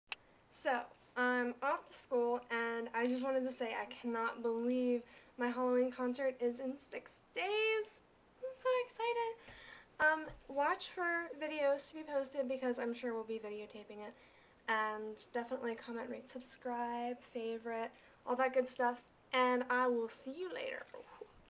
My concert is in 6 days!!!!!
I am soooo excited to be performing my first concert EVER on Halloween!!! I'm going to be doing all covers of some pretty awesome Halloween songs and some that I just really wanted to sing :) Check in for videos from the performance!!!